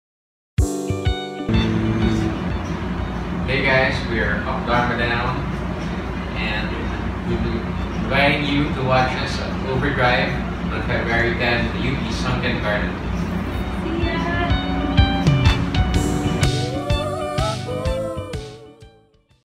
Up Dharma Down on Overdrive UP Fair 2016
1 day left 'til Up Dharma Down rocks the stage on UP Sunken Garden for Overdrive UP Fair 2016 this Wednesday, February 10! See you there!